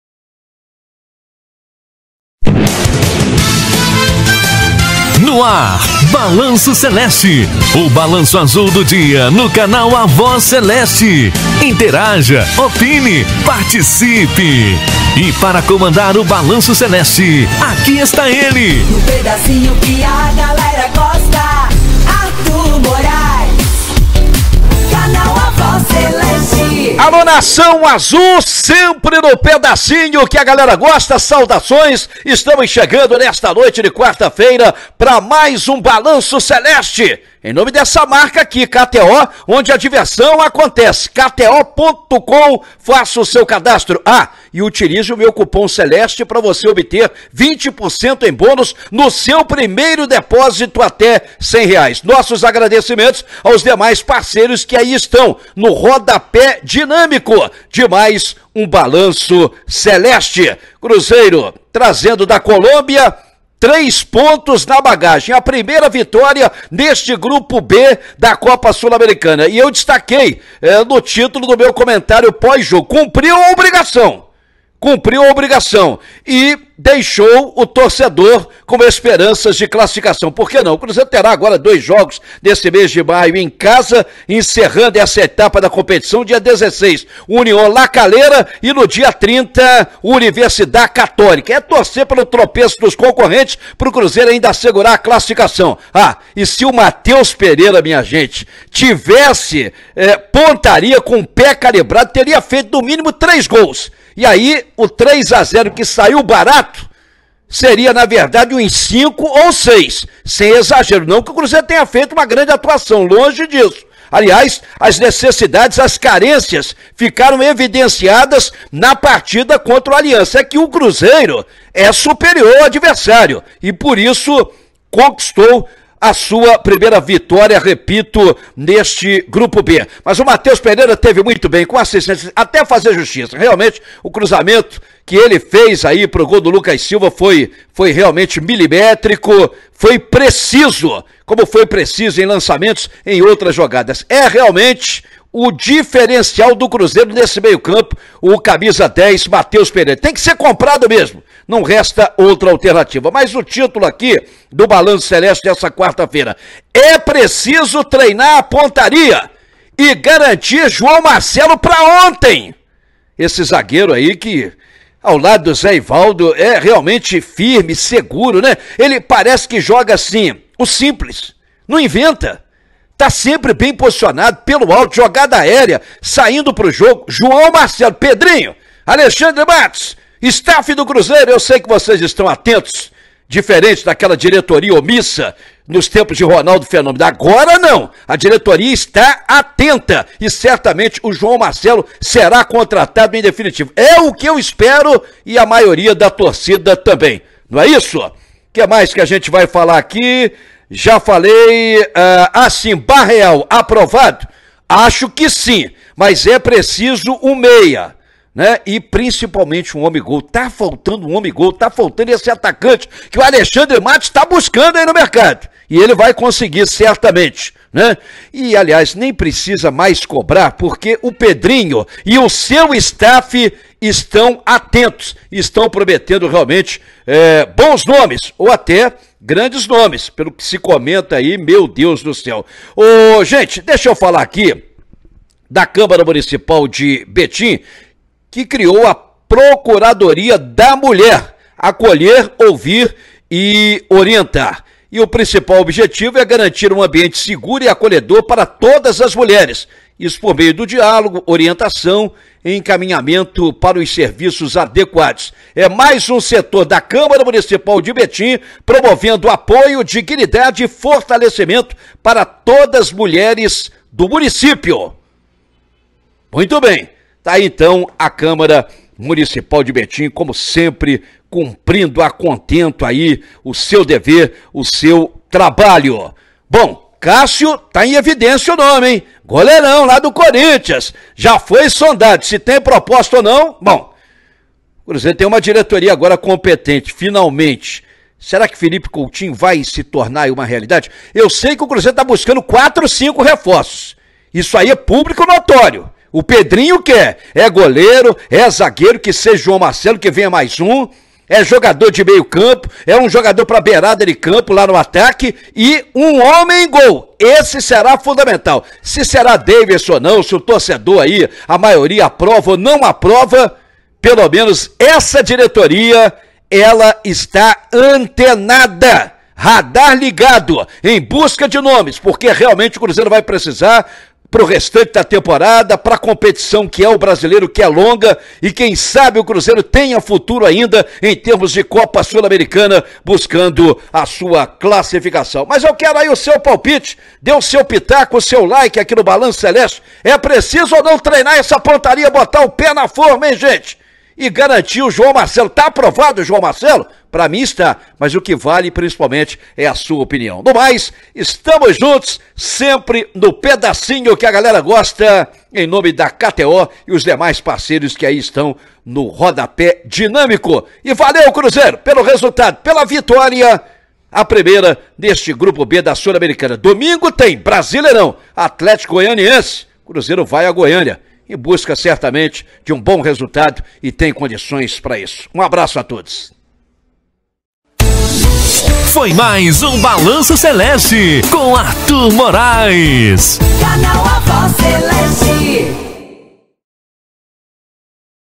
BALANÇO CELESTE | Não precisa cobrar, Pedrinho e diretoria estão monitorando nomes!
O que você acha, torcedor? Deixe sua opinião.

Balanço Celeste de segunda a sexta.

 • BALANÇO CELESTE

KTO - ONDE A DIVERSÃO ACONTECE: Use o Cupom CELESTE e ganhe um bônus de primeira recarga.